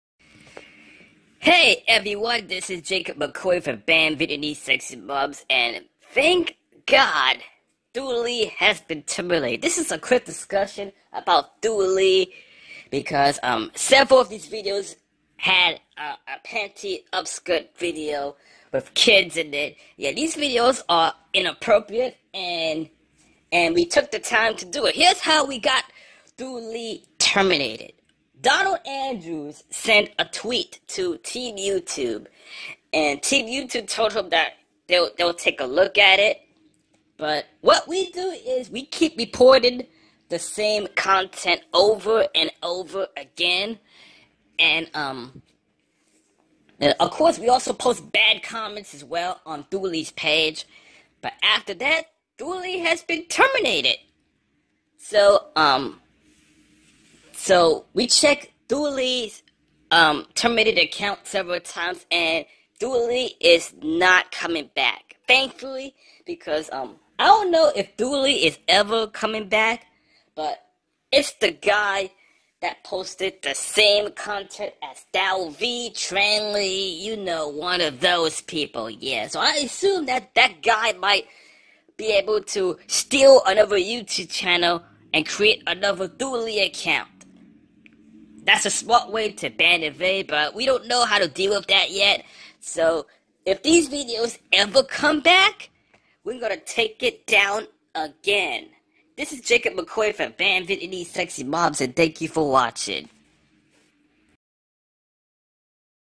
YouTube TERMINATES Thuy Lee
Team YouTube did the right thing by terminating this channel after 4 panty-upskirt videos featuring minors within a video. Also, he often phishes people's E-mail and the same applies with Ha Trangg, Trang Lee and Thao Vy.

Ratings is disabled and comments are being moderated before approval.

EDIT: I have to blur parts of my video because the video did somehow got taken down! I am doing my best to prevent my channel from taking down and ALL CONTENT IS COMMENTARY BASED!